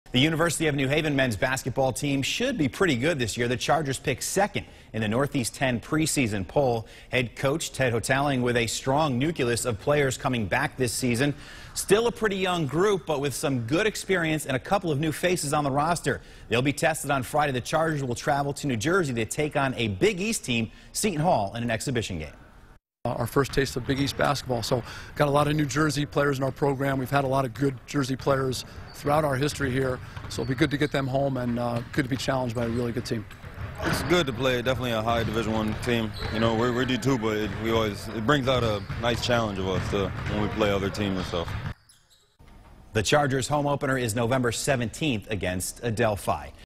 UNH men's basketball picked second in preseason poll
The University of New Haven men's basketball team should be pretty good this year.